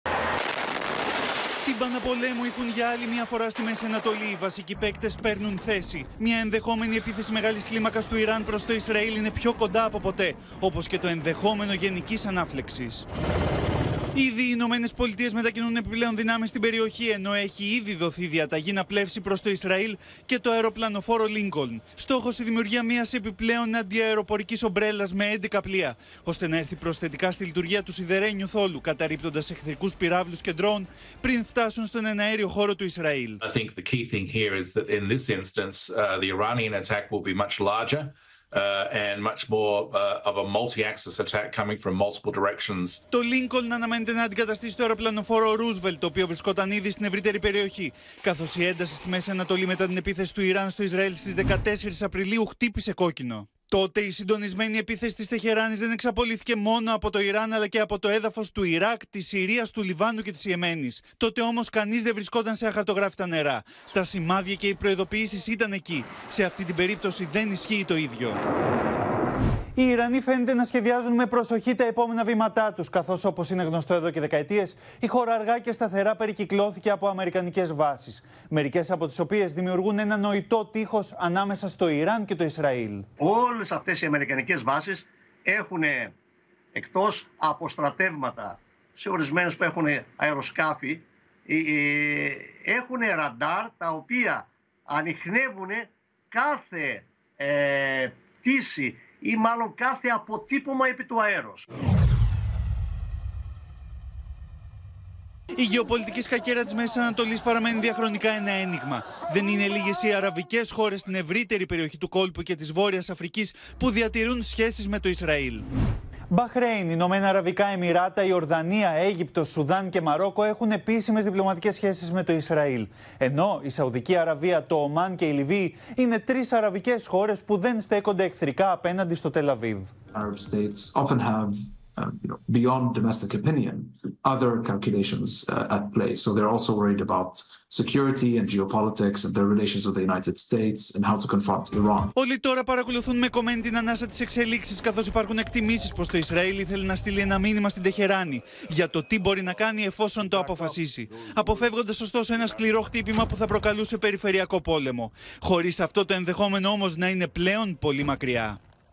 Τύμπανα πολέμου στη Μέση Ανατολή | OPEN TV
Γίνετε μέλος στο κανάλι του OPEN για να αποκτήσετε πρόσβαση σε προνόμια:
Ενισχύουν τις δυνάμεις τους στην περιοχή οι Αμερικανοί.